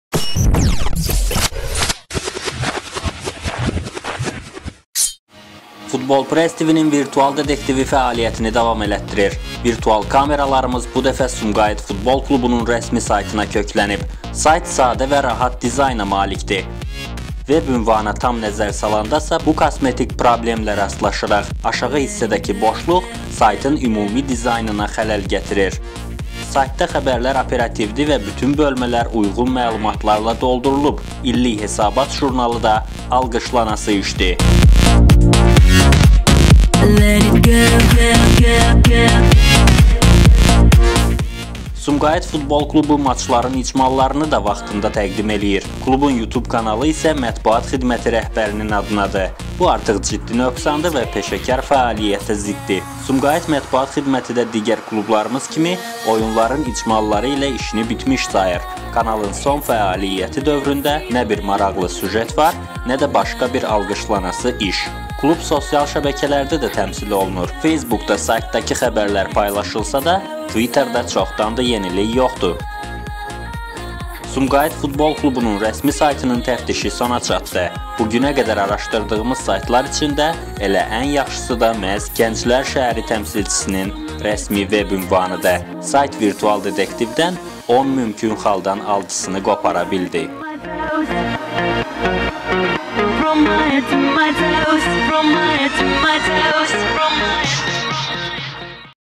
FutbolPress TV: Virtual Detektiv - "Sumqayıt"ın rəsmi saytındakı deffektlər
Hazırladı: Elvin CanTuran
Baş redaktor: Elçin Cəlilov